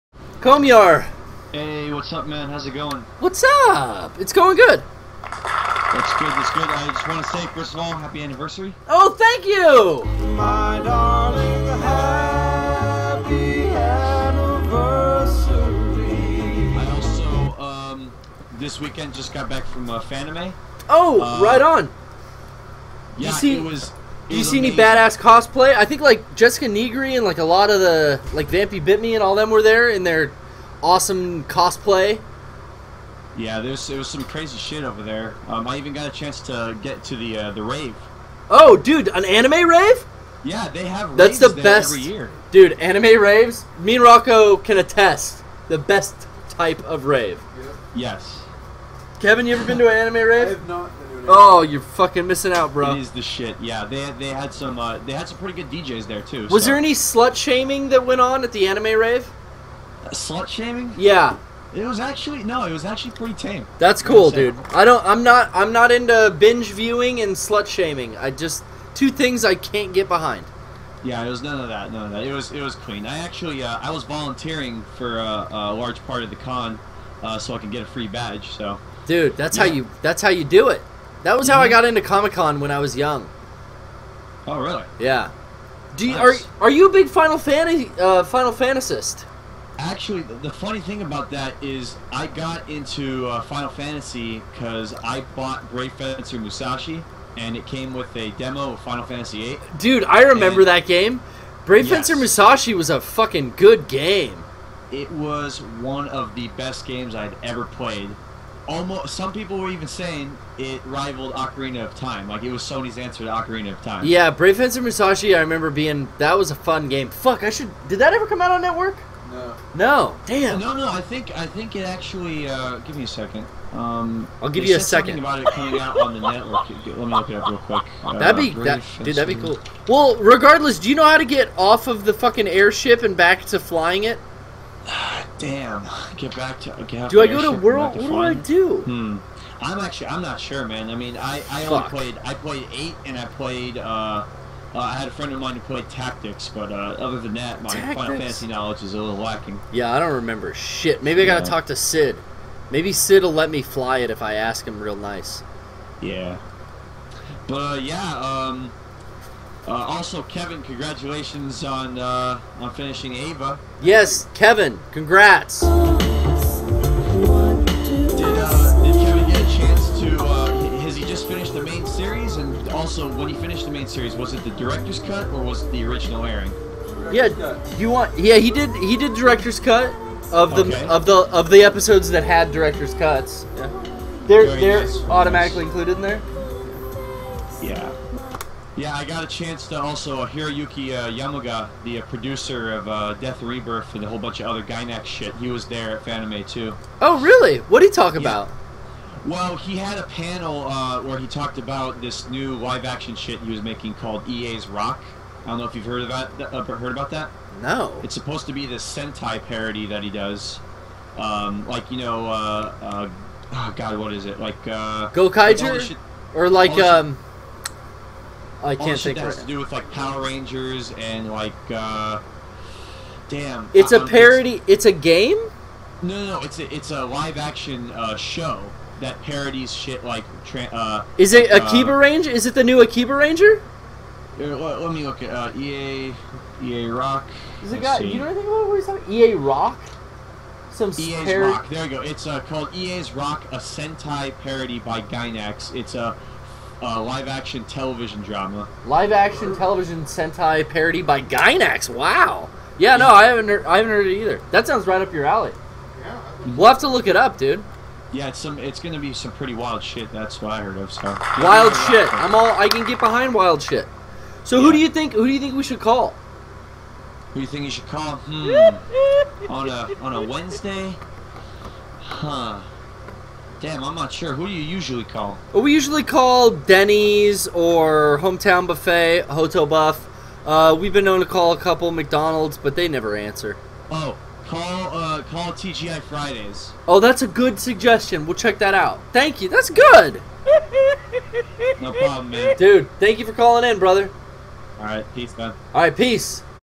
Mega64 Poorly Played Stream 49 - Kamyar's Call
Kamyar calls in to congratulate Garrett on his first year of the Poorly Played Stream.